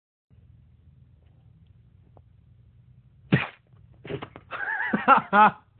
Michael And Sister
Michael playing with our cat